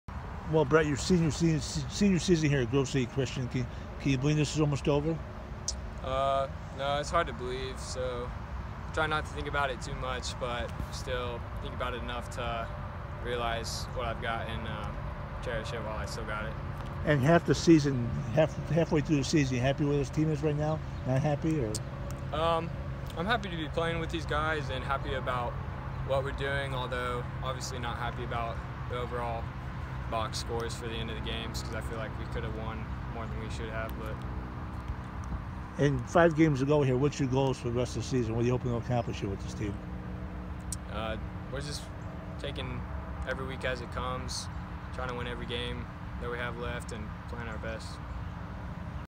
Grove City Christian’s Brett Baker
Grove City Christian football player Brett Baker talks about his senior season. [Frank DIRenna/ThisWeek]